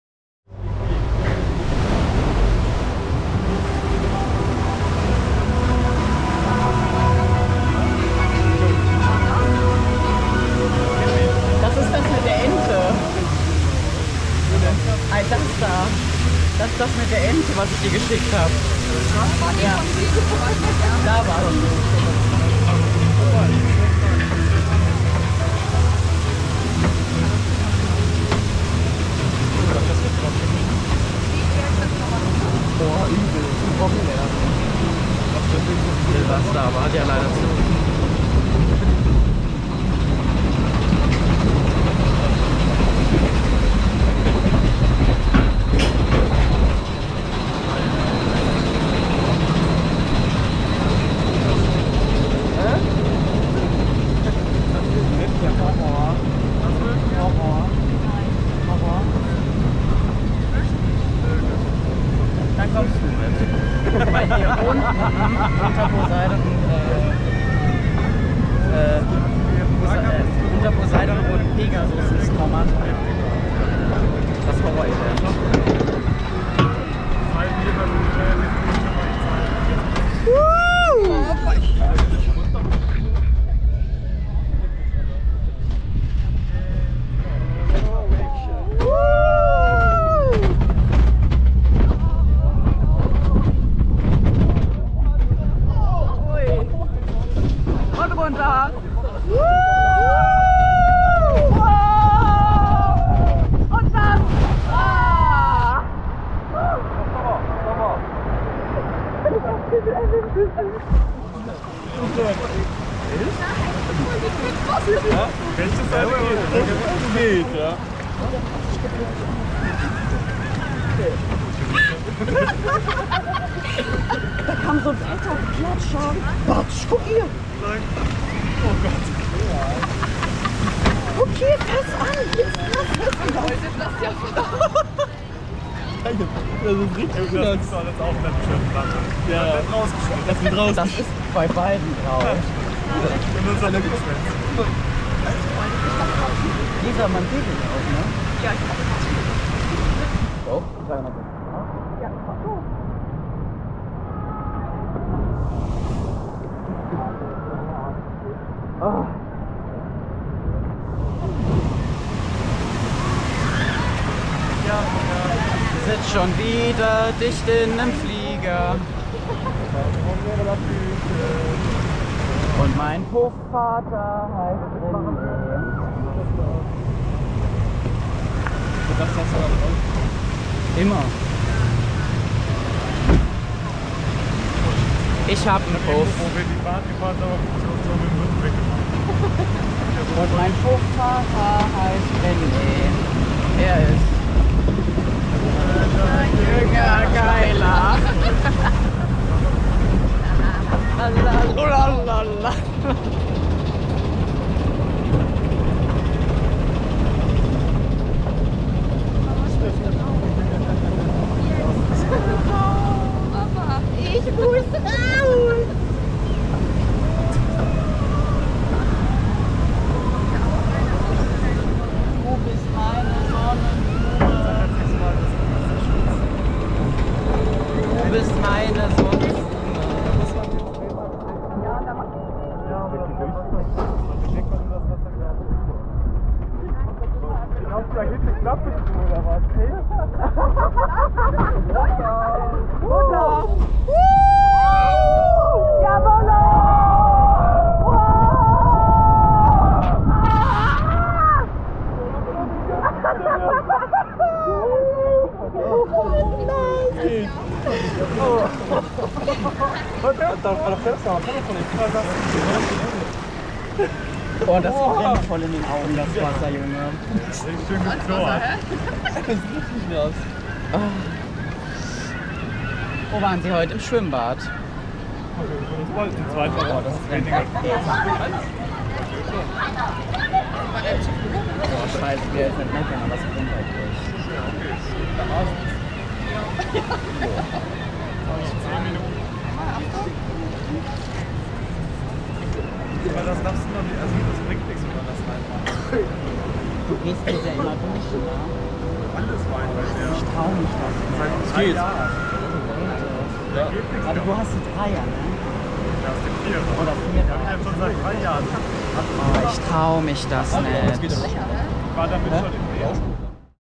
Poseidon - by Night (Onride) Video Europa-Park Rust 2022
Poseidon - by Night (Onride) Video von der Sommernachtsparty im Europa-Park in Rust bei Freiburg 2022.